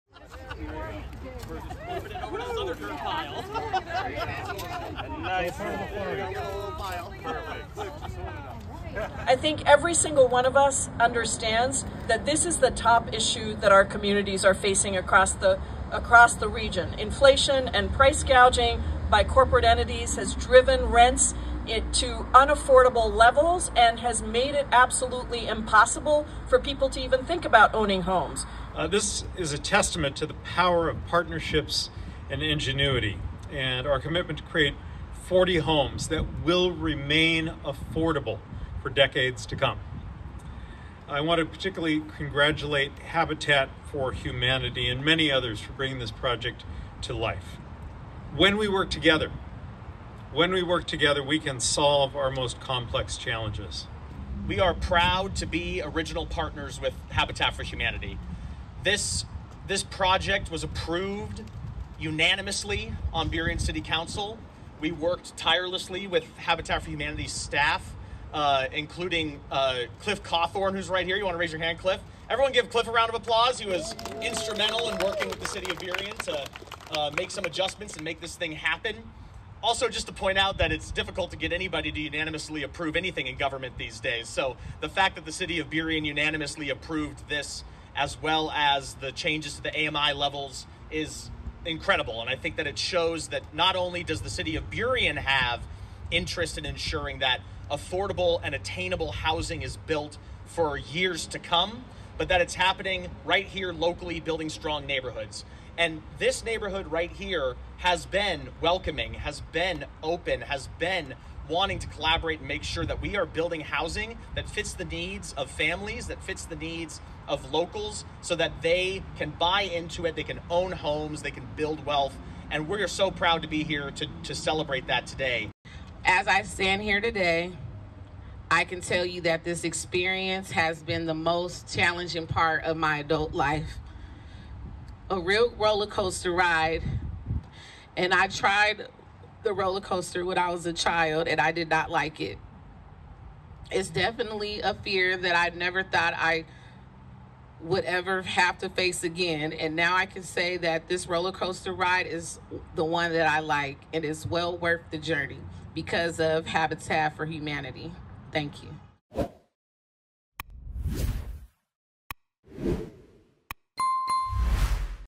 Groundbreaking for Habitat for Humanity Miller Creek Community 8/13/24
On Aug. 13, 2024, a groundbreaking ceremony was held for the new Habitat for Humanity Miller Creek Community affordable housing development in Burien, WA. This event featured Congresswoman Pramila Jayapal, King County Executive Dow Constantine, Burien Mayor Kevin Schilling and new homeowner Racquet G.